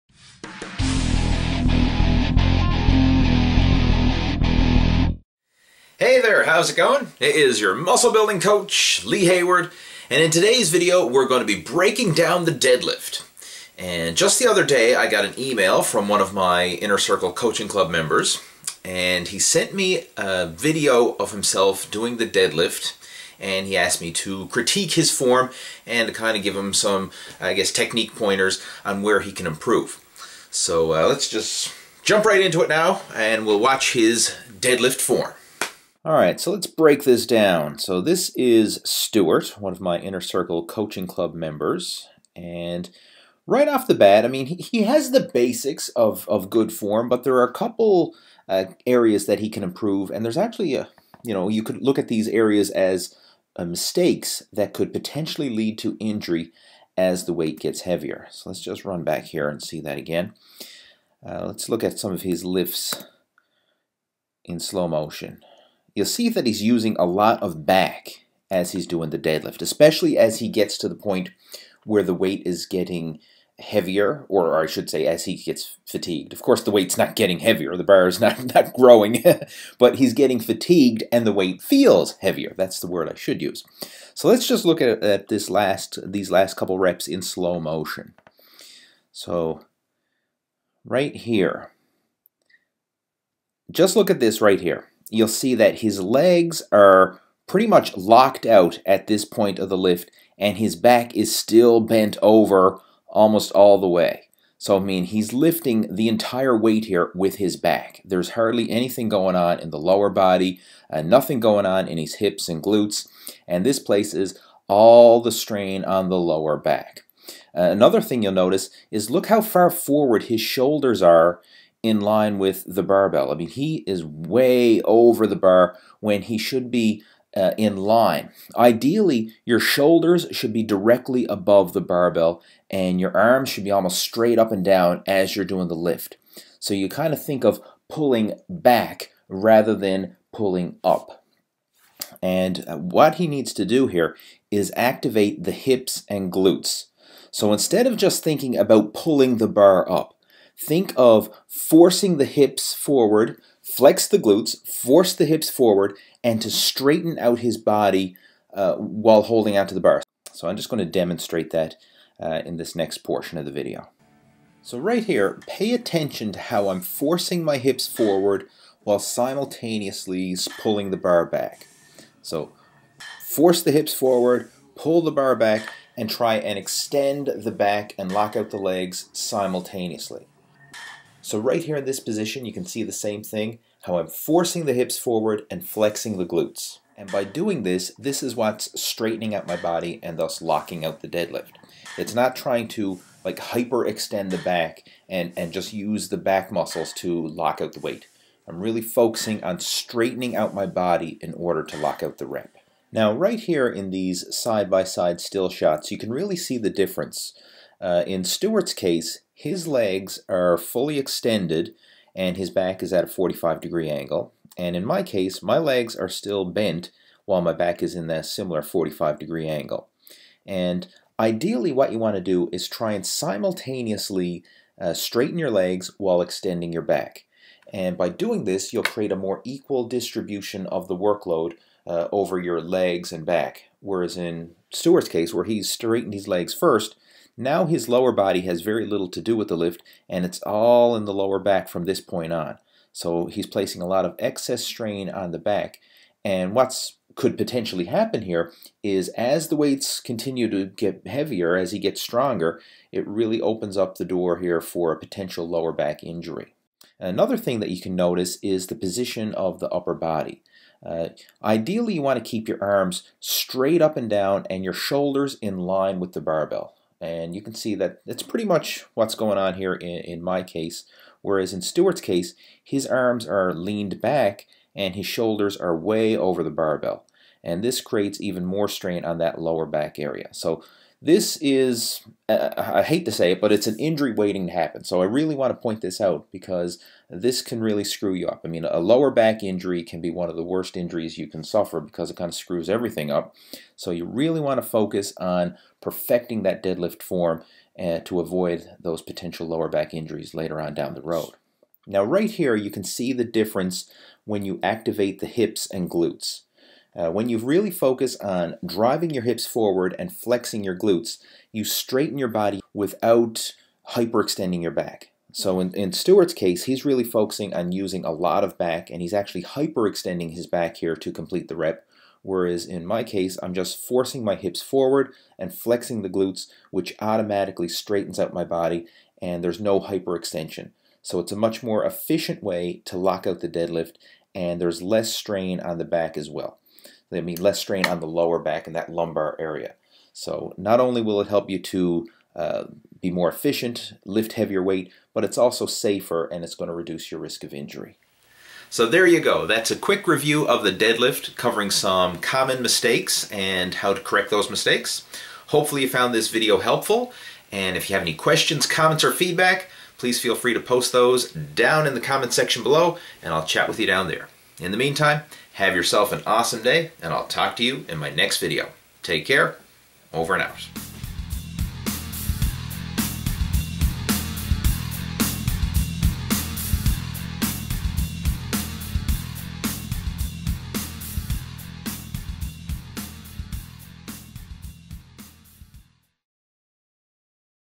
Deadlift Form Critique - detailed video breakdown...
Breaking Down The Deadlift & Outlining Some Common Mistakes.

Just recently one of my Inner Circle Coaching Club members sent me a video of himself doing the deadlift and he asked for my opinion, insights, and some constructive criticism of his lifting form.

So that's exactly what we're going to cover in this deadlift technique video breakdown!

I'm going to outline some critical deadlift technique mistakes and show you how to fix them. This will allow you to lift heavier weights, with better technique, while reducing your risk of injury.

If you would like more info about proper deadlift technique, I recommend that you check out my deadlift 101 blog post at: